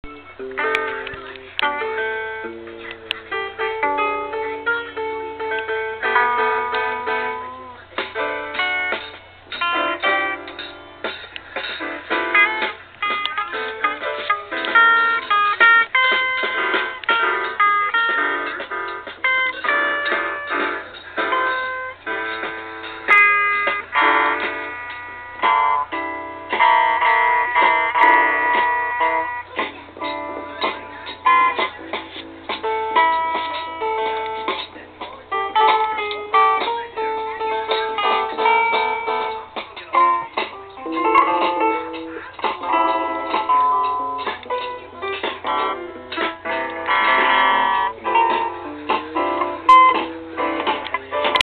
Worst Band Ever
My sims formed a band, but sadley they are horrible. They call theirselves 'The Best Band Ever'. Bigfoot is on guitar, Barbie on bass, Alien on drums and Evil Clown is on piano. There was a shemale pink dragon singing, but who knows what happened to him. Enjoy!